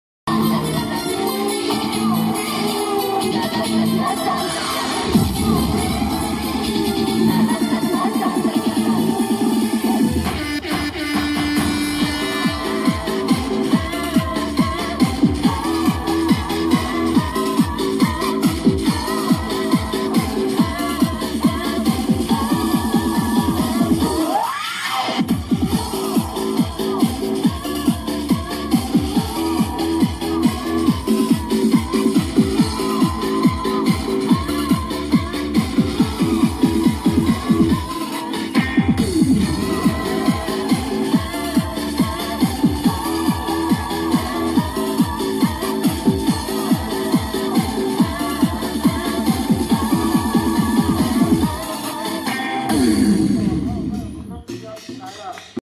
2024年歲次甲辰年 四月八永貞宮客家媽祖巡庄活動 舞台表演 17-1 Hot Q girls
May 16, 2024
其他影片不適合在 YouTube 上播放的影片 請前往：